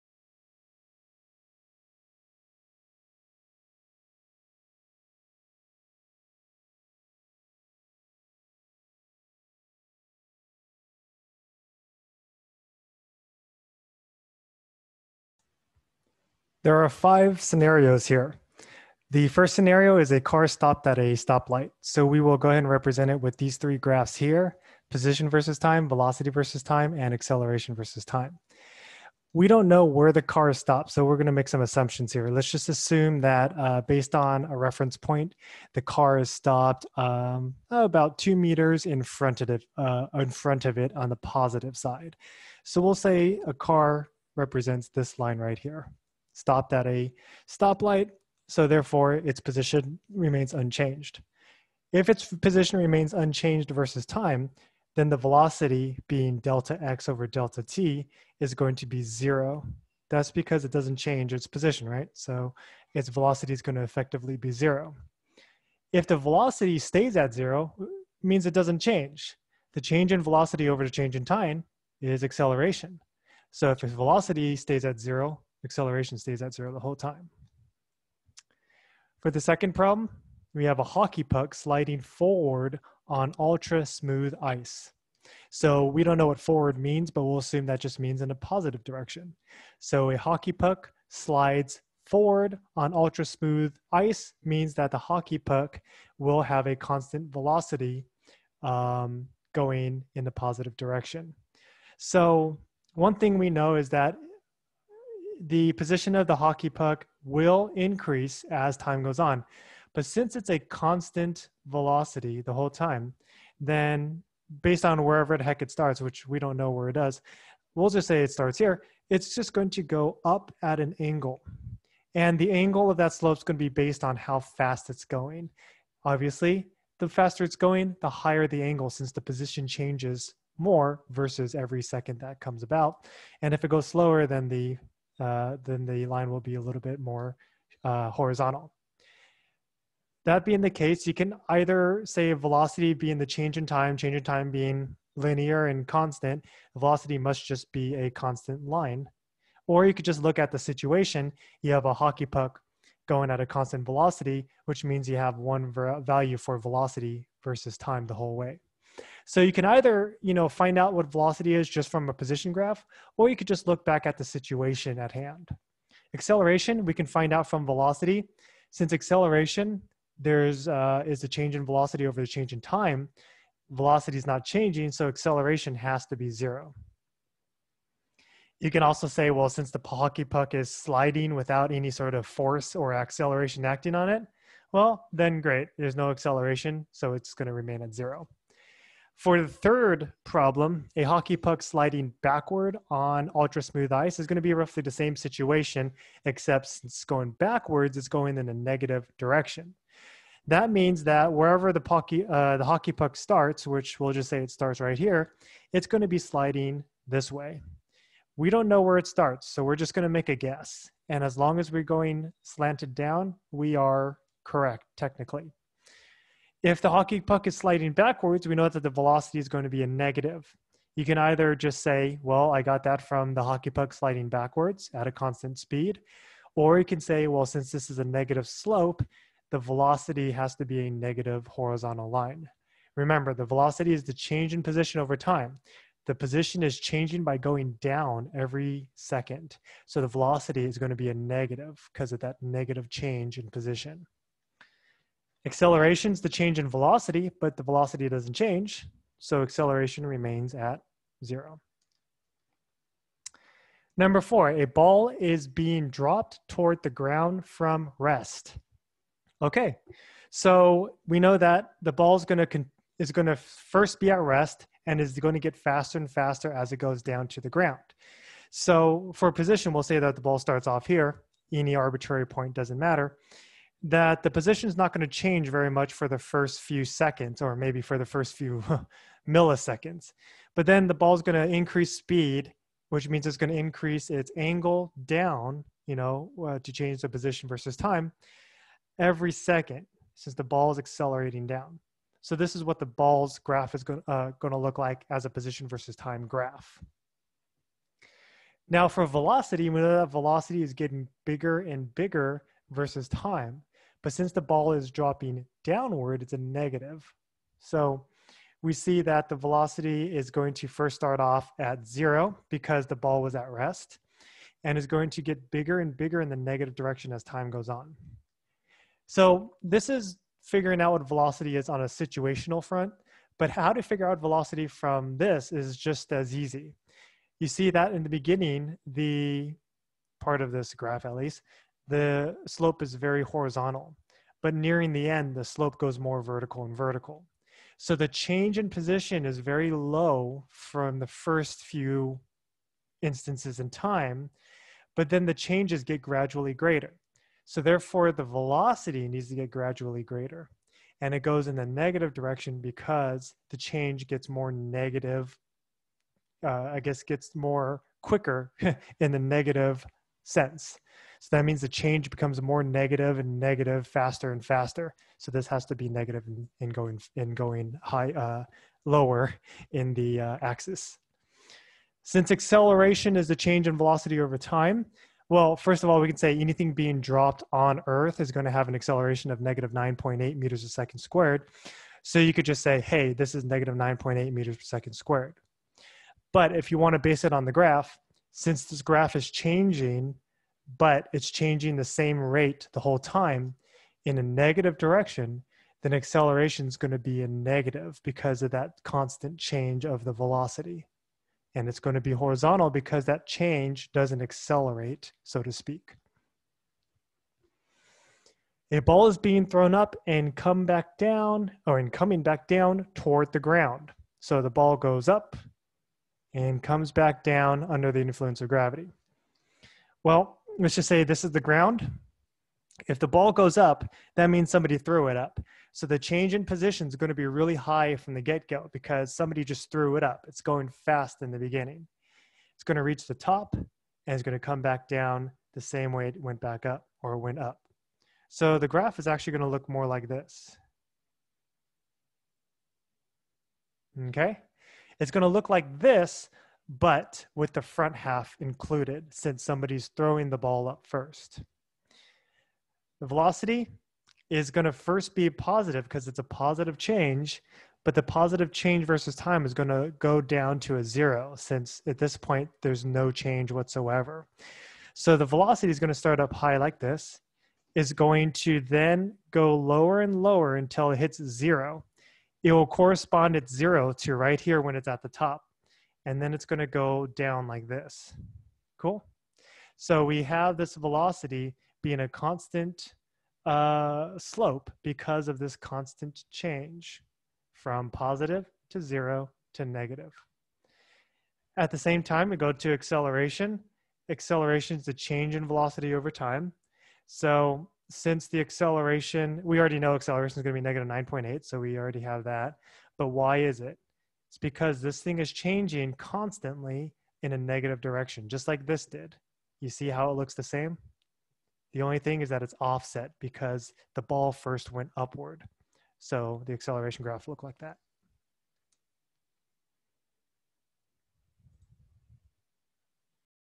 Graphing position, velocity, and acceleration versus time for different situations
What does a graph of position/velocity/acceleration versus time look like for the following situations?
1) A car at a stop light
2) A hockey puck sliding forward on ultra-smooth ice
3) A hockey puck sliding backward on ultra-smooth ice
4) A ball being dropped toward the ground from rest
5) A ball being thrown up and coming back down toward the ground